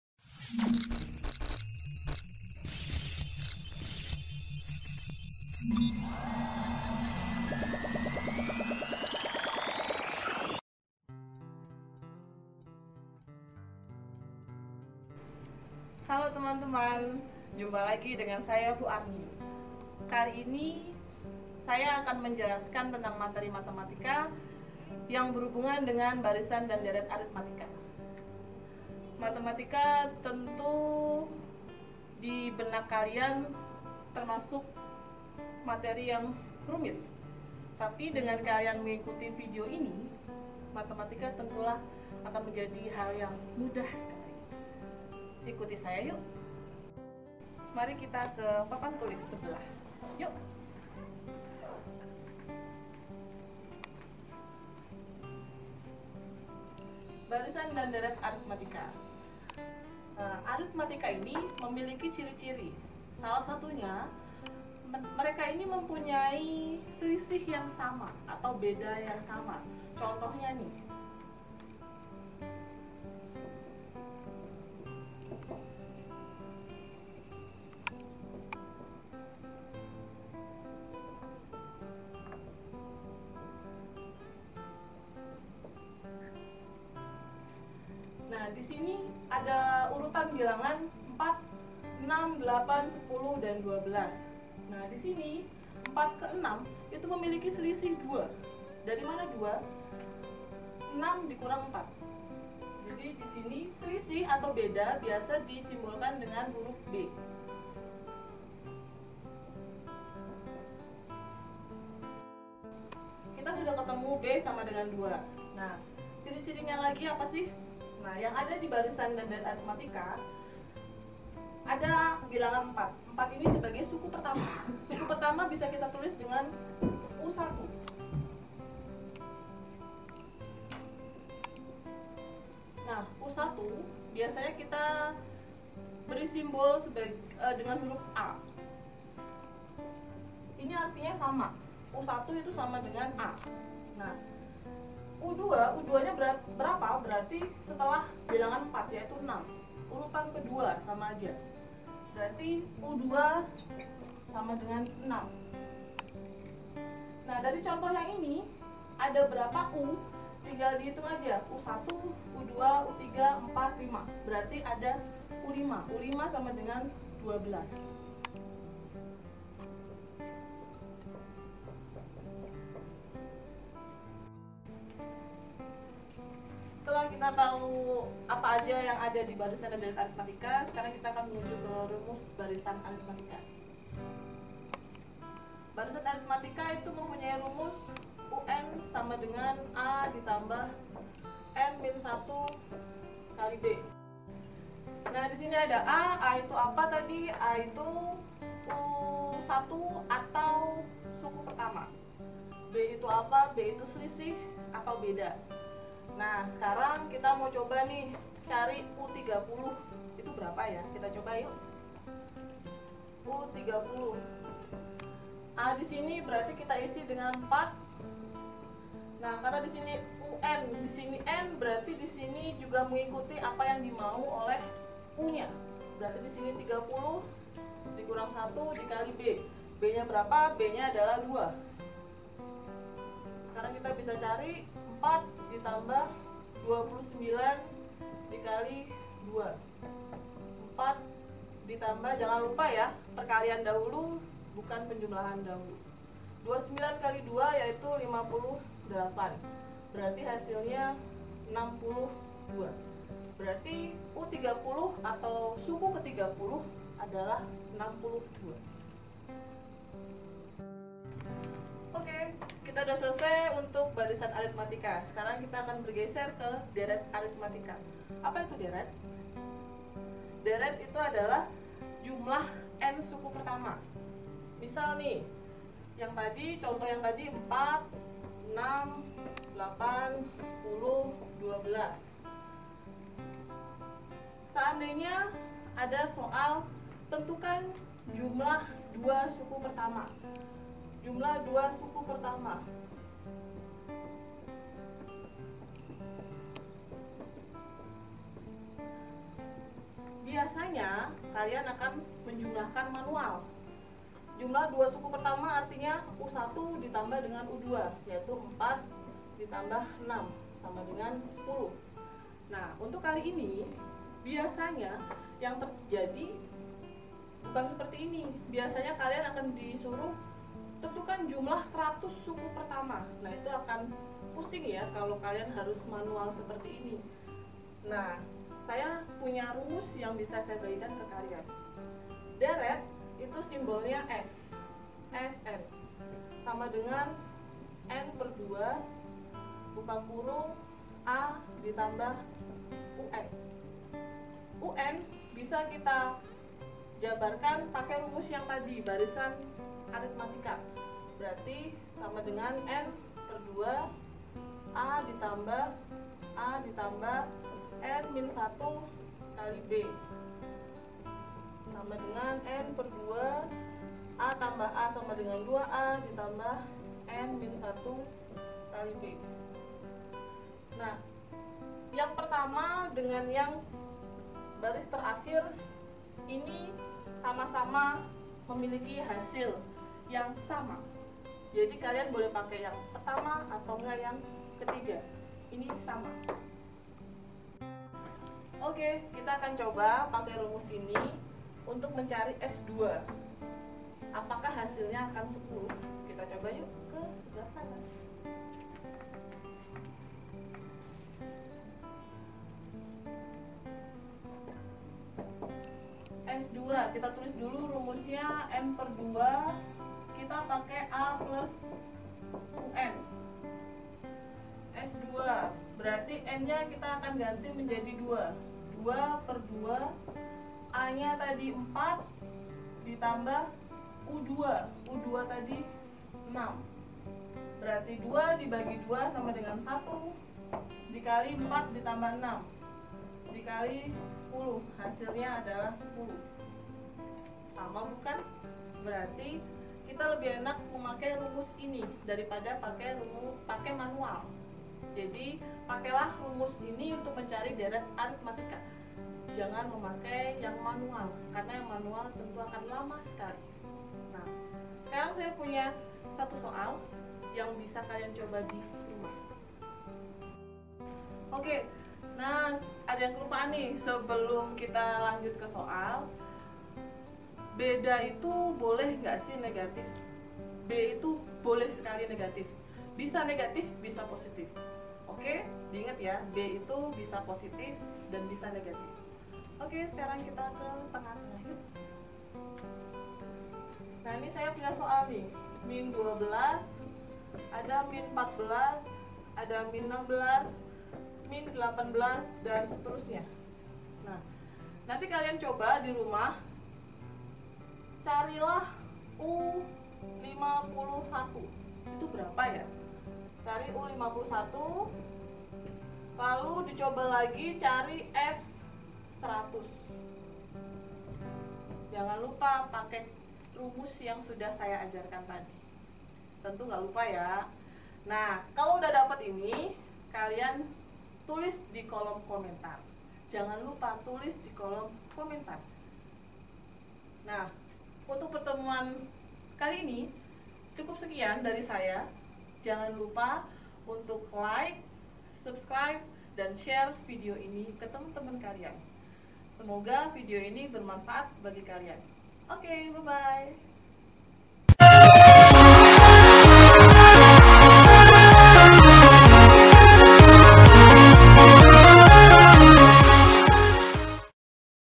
Barisan dan Deret Aretmatika
Kami persembahkan video ini untuk para peserta didik kami. Materi Matematika kelas 11 di sampaikan oleh Ibu Agustina Purgo Artonia, S.Pd atau lebih dikenal dengan nama Bu Arni. Salah satu staff pengajar di SMA Pangudi Luhur Bernardus Deltamas.

background song